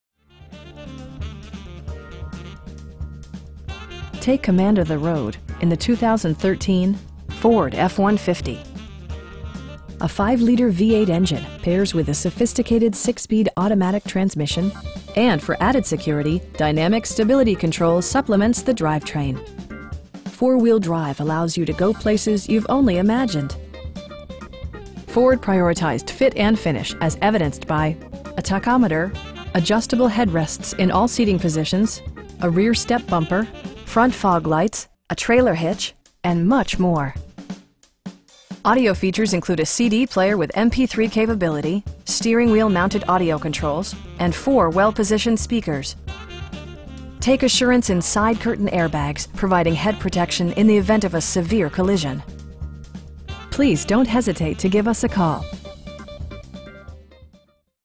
2013 Ford F-150 XLT in Buford, GA 30518
4525 Nelson Brogdon Blvd.

Take command of the road in the 2013 Ford F-150. A 5 liter V-8 engine pairs with a sophisticated 6 speed automatic transmission, and for added security, dynamic Stability Control supplements the drivetrain. Four wheel drive allows you to go places you've only imagined. Ford prioritized fit and finish as evidenced by: a tachometer, adjustable headrests in all seating positions, a rear step bumper, air conditioning, front fog lights, a trailer hitch, and much more. Audio features include a CD player with MP3 capability, steering wheel mounted audio controls, and 4 speakers, providing excellent sound throughout the cabin. Take assurance in side curtain airbags, providing head protection in the event of a severe collision. We offer competitive pricing, and the area's best shopping experience. Please don't hesitate to give us a call.